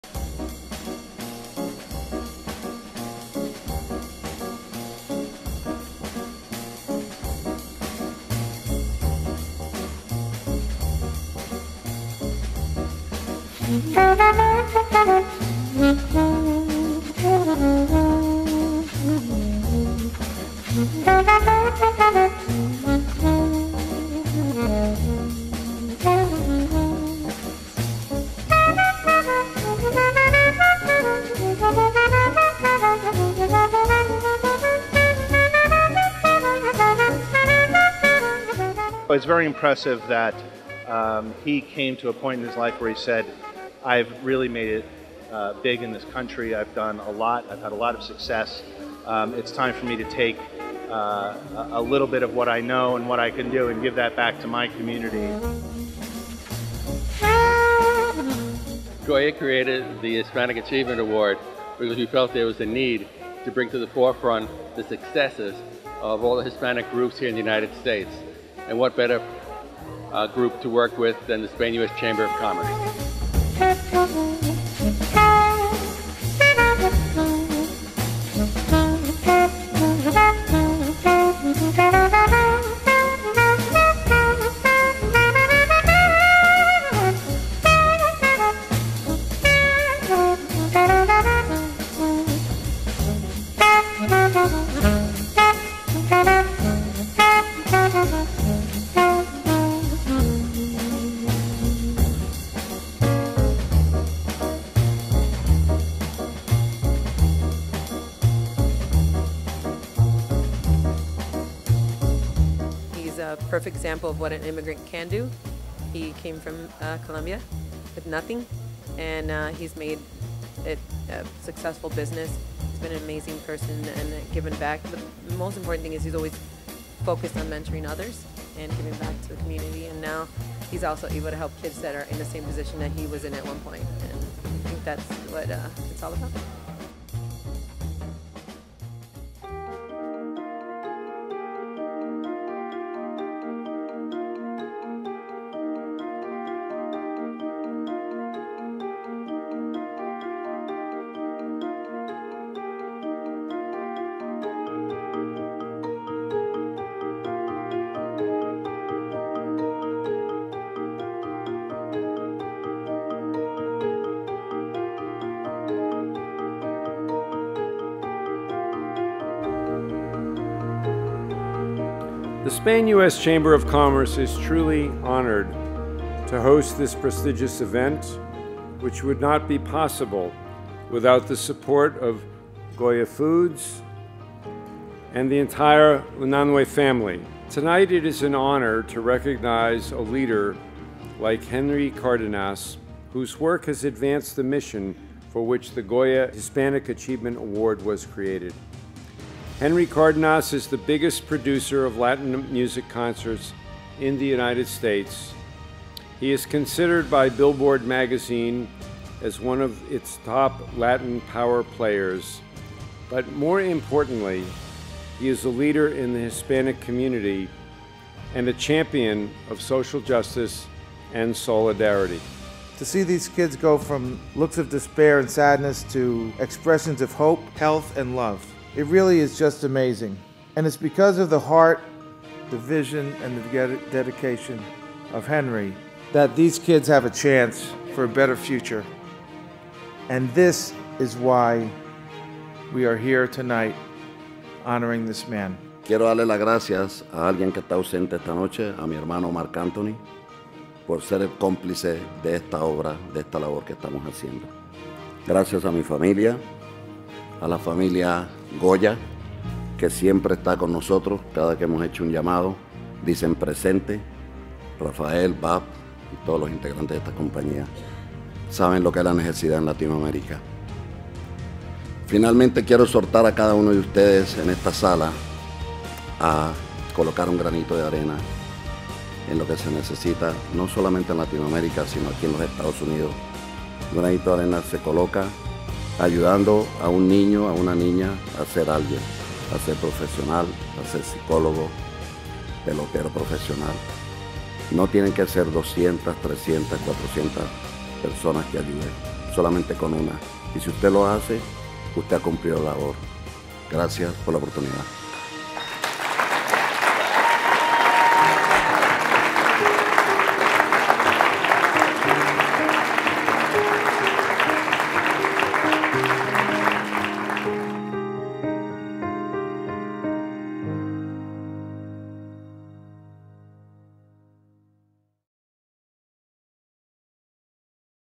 2017 - III Goya Hispanic Achievement Award
Goya Foods, the largest Hispanic-owned food company and the Spain-U.S. Chamber of Commerce honored Henry Cárdenas with the III Goya Hispanic Achievement Award at an exclusive gala dinner at the Rainbow Room on Monday, May 15, 2017.